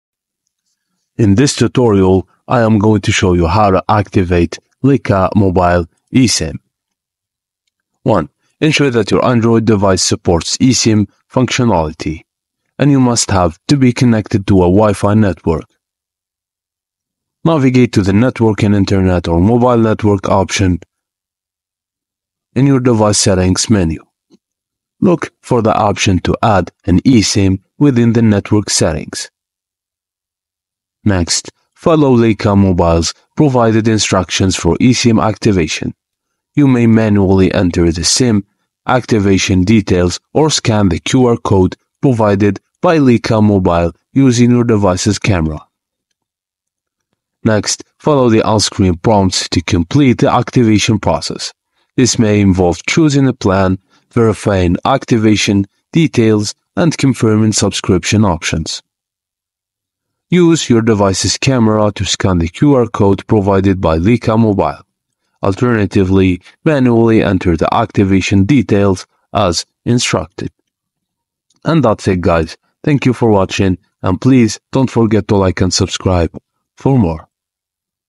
How To Activate Lyca Mobile Esim (2025) STEP BY STEP
How To Activate Lyca Mobile Esim.
To activate a Lyca Mobile eSIM, you'll need a compatible device, a stable internet connection, and your eSIM activation details, which include a QR code or activation information. After purchasing your eSIM and receiving the activation details, scan the QR code with your phone's camera (or enter the details manually in settings) and follow the on-screen prompts to install and activate the eSIM